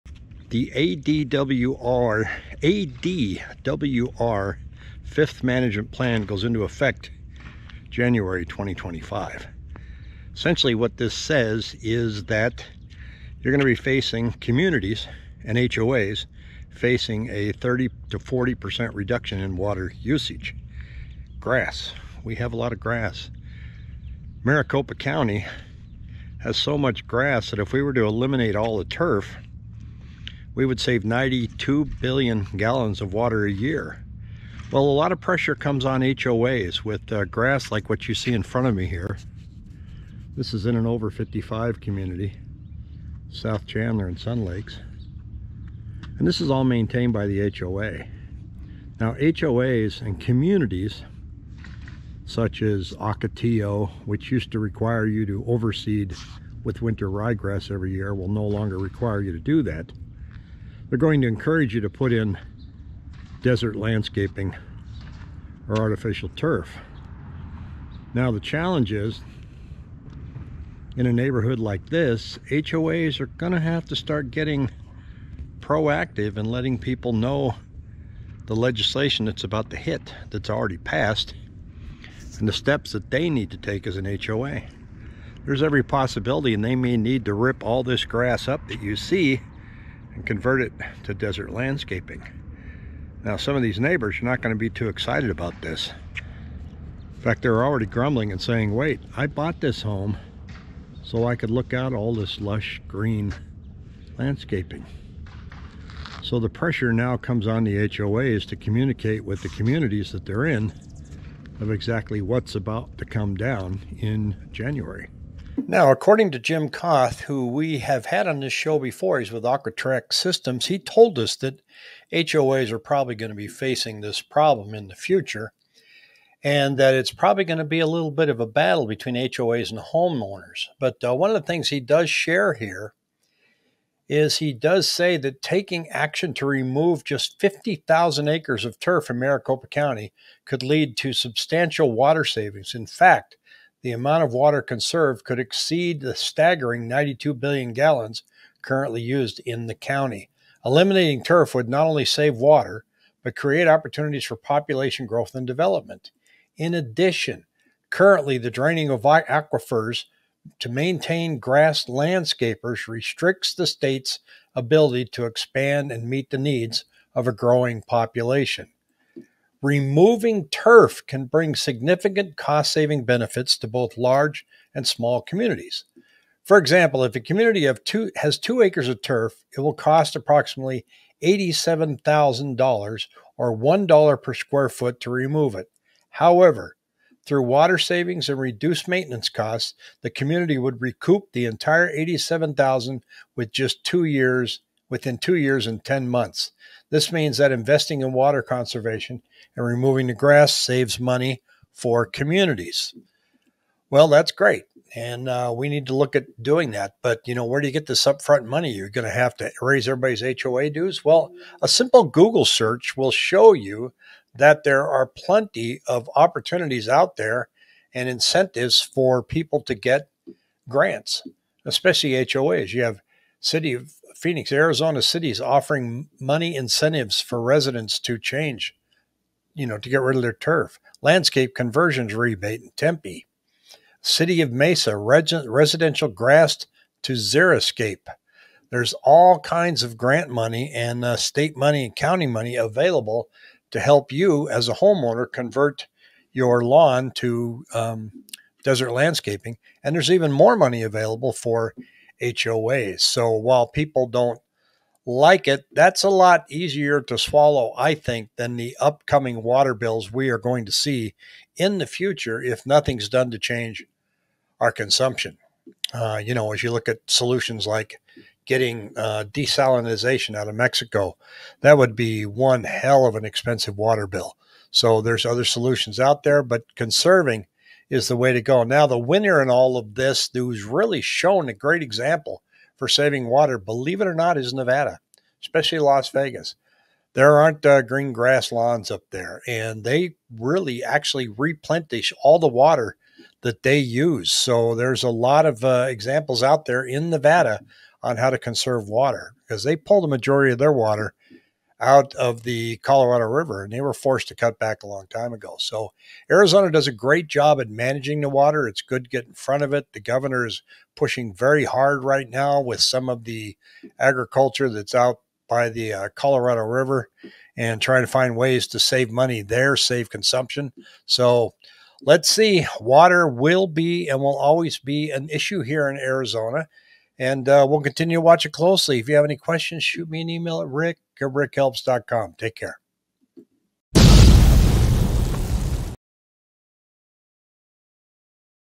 Arizona's Water Crisis | Get Ready...
Water cuts are coming in January 2025 that will affect homeowners that are in an hoa.

Referral Partners:
Lending:

GNXMMBSH93977054